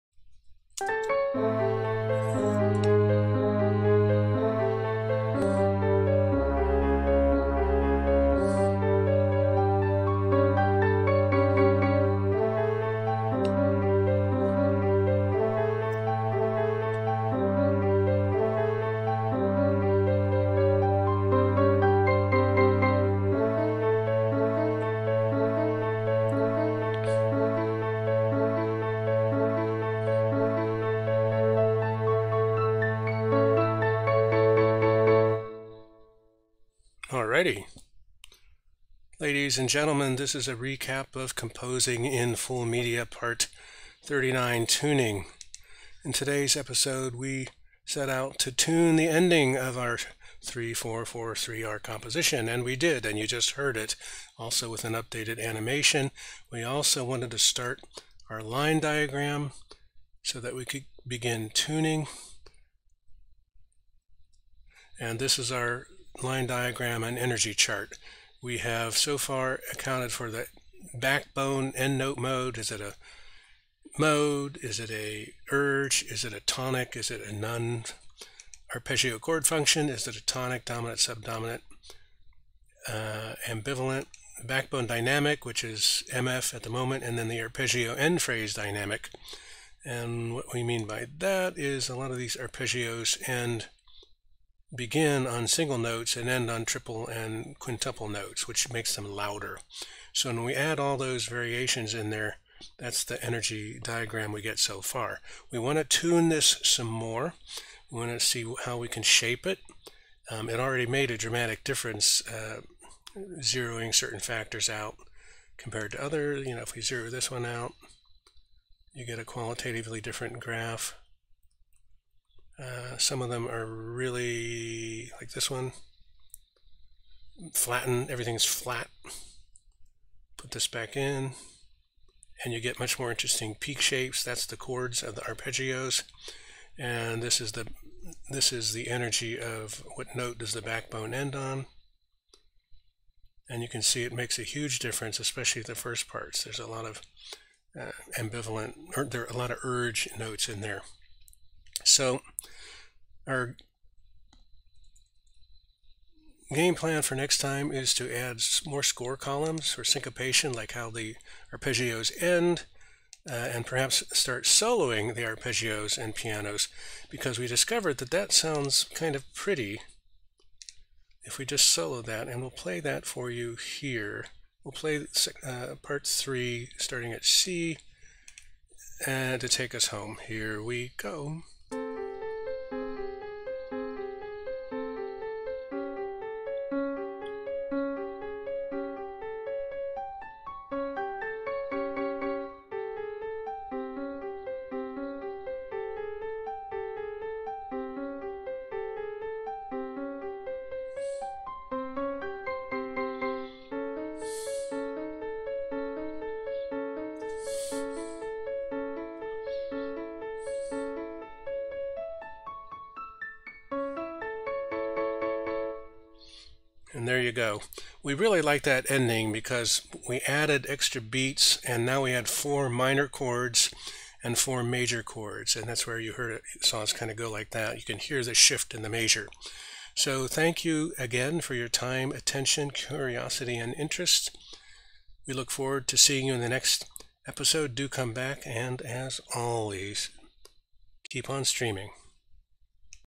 Composing in Full Media Part 39 TUNING Stream Highlight 09 Oct 2020
the ending is strengthened and added to the animation and a line diagram and energy chart are begun